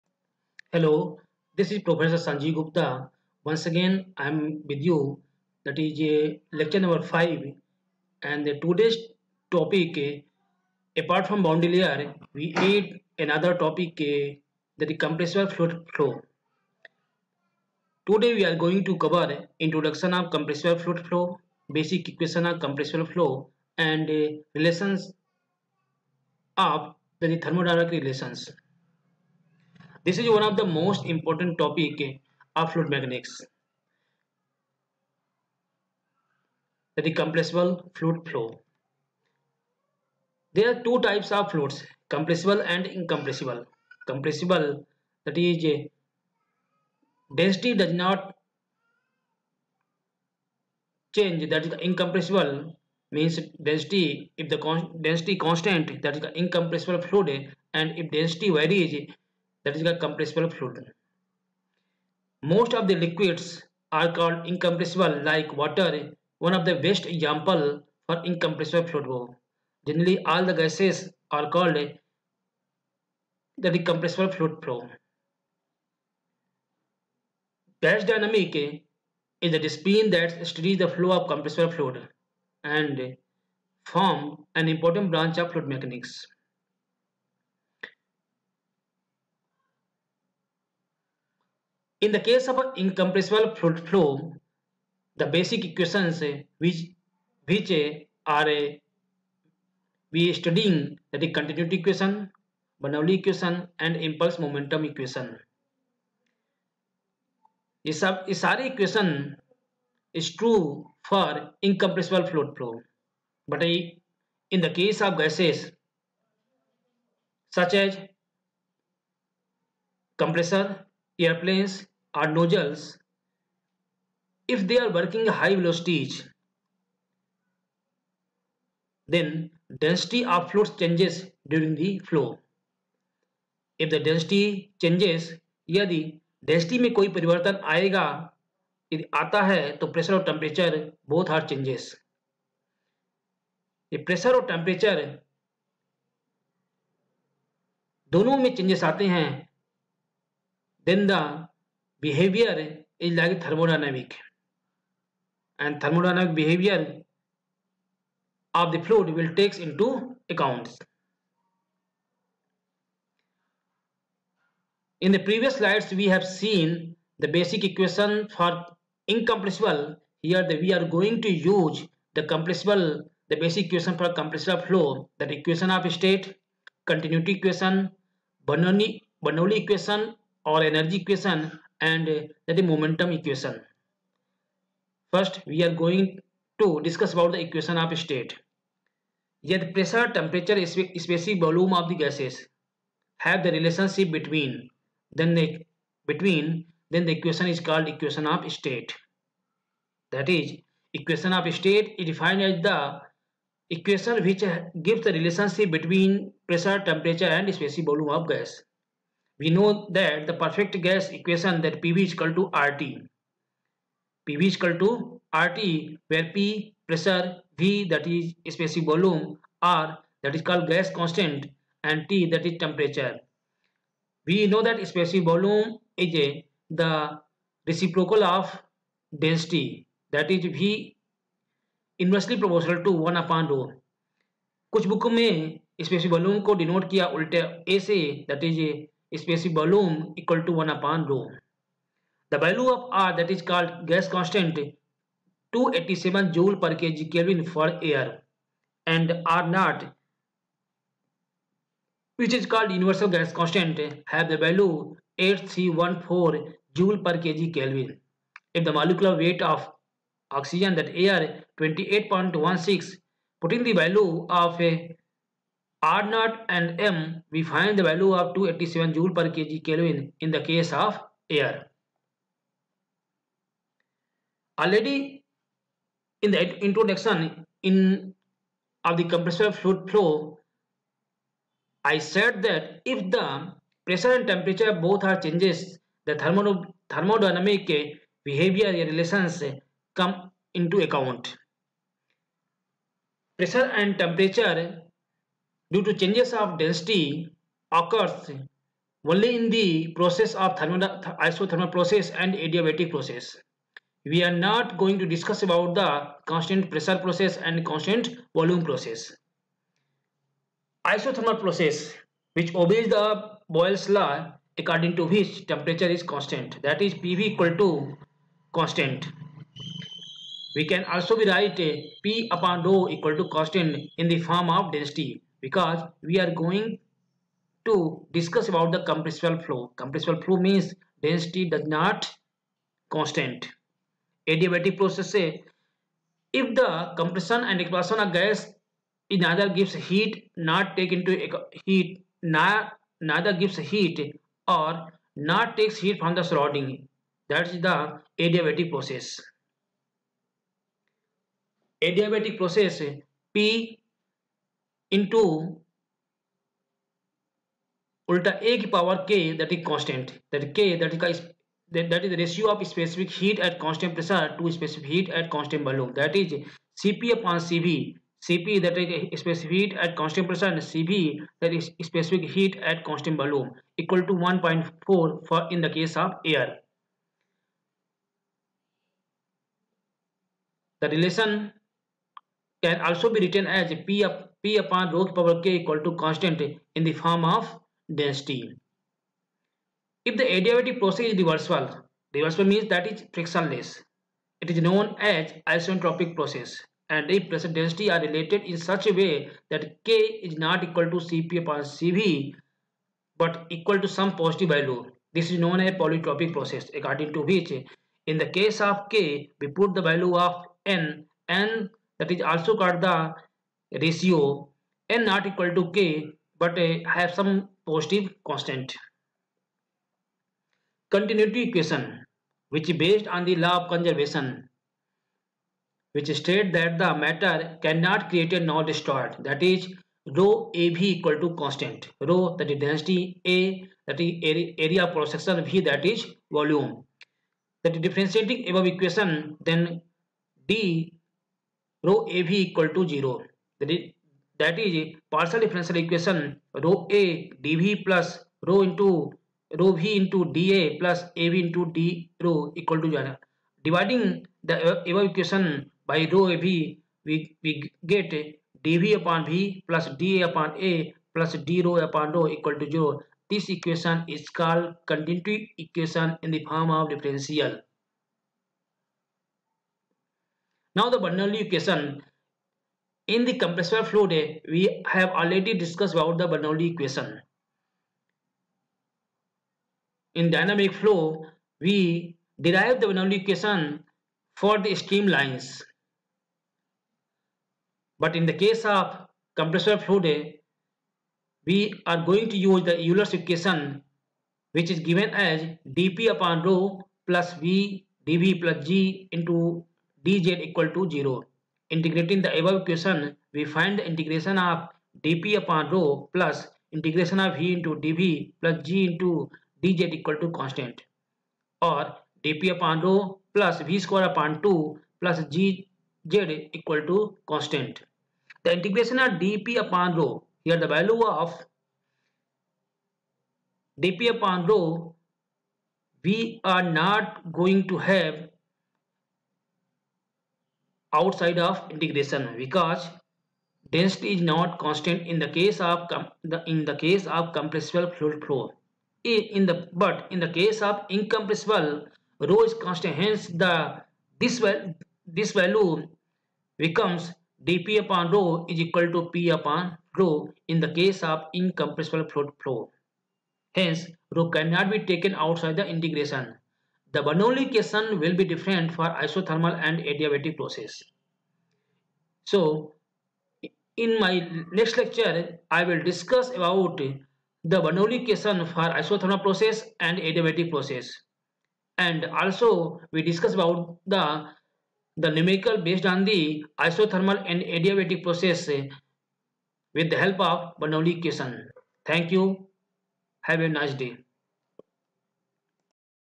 Compressible Fluid Flow|Prof Sanjeev Gupta|Depertment of Mechanical Engg SISTec-Ratibad Bhopal
Introduction of Compressible Fluid Flow
Basic equation of compressible flow
Thermodynamic Relations